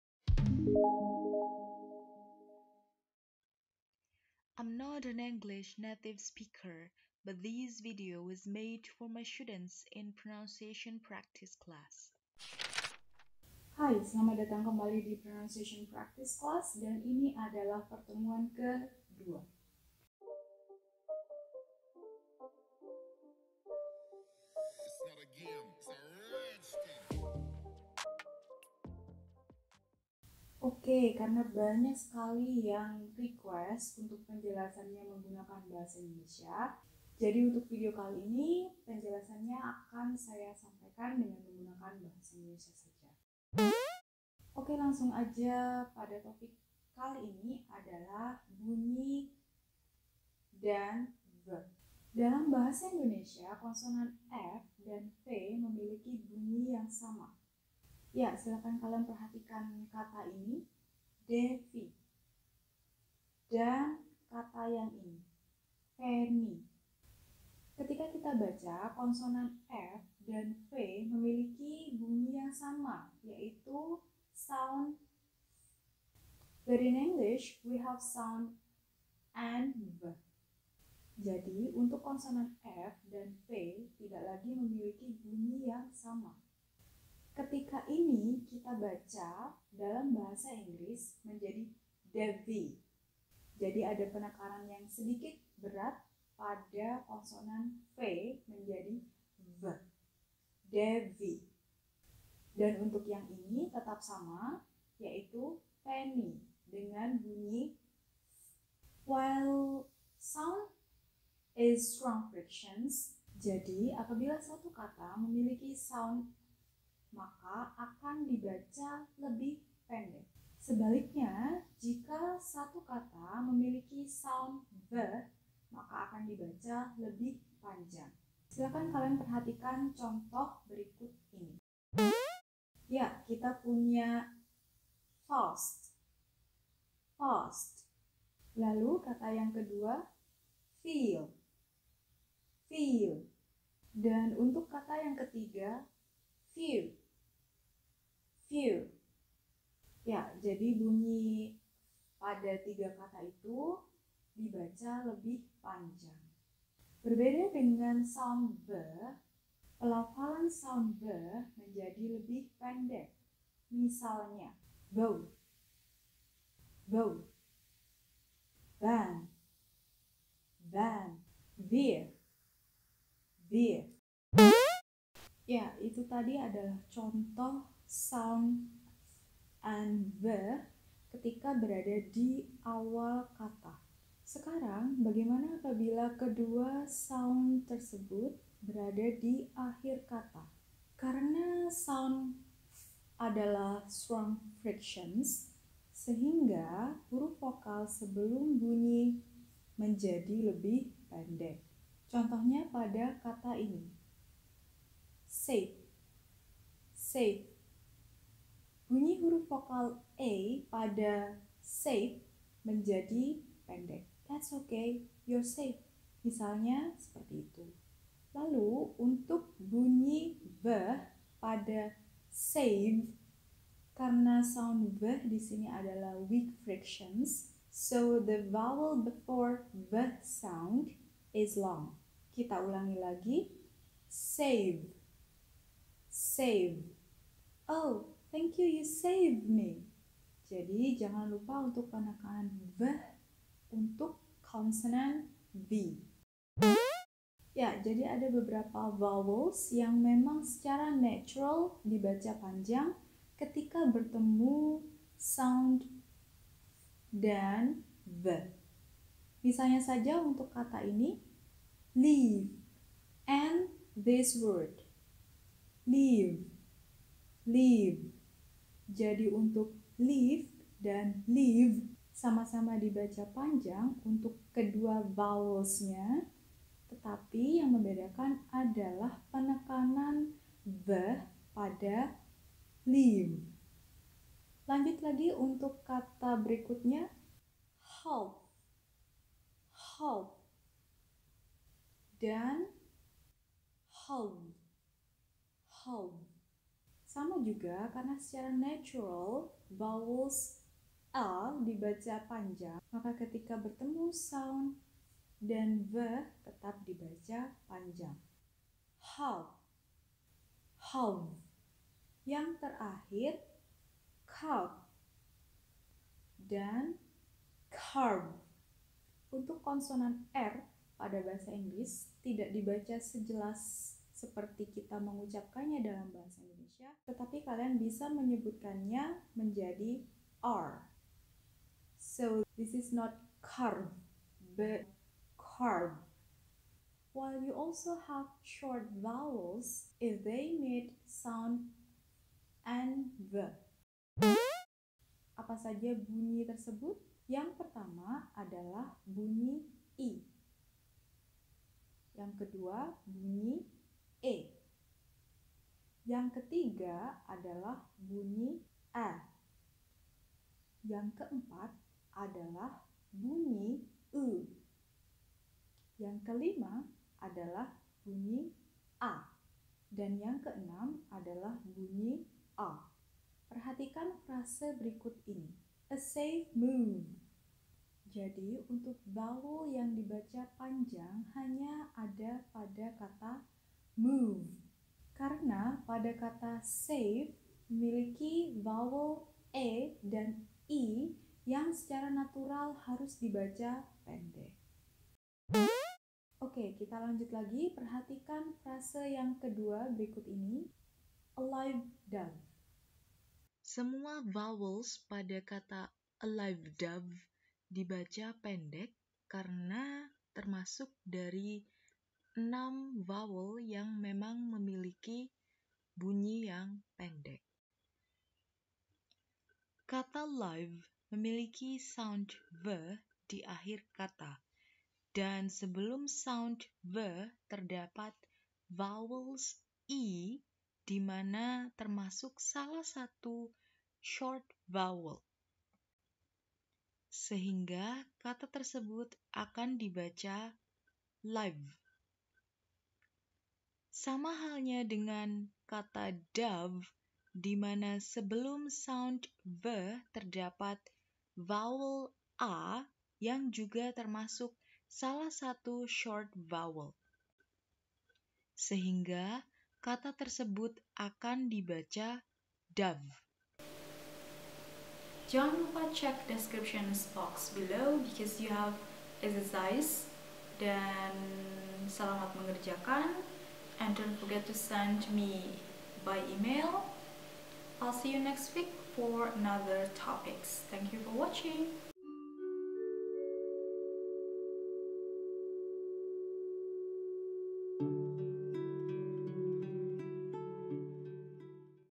VOCABULARY AND PRONUNCIATION PRACTICE #2
Practice these words and record your voice.
you have a week to submit your assignment (before 3rd meeting posted). Have fun!
1. Fan – Van
2. File – Vile
3. Ferry – Very
4. Proof – Prove
5. Surf – Serve
6. Ralph – Rave
7. Strife – Strive
8. A grave grief
9. A cliff drive
10. A brief love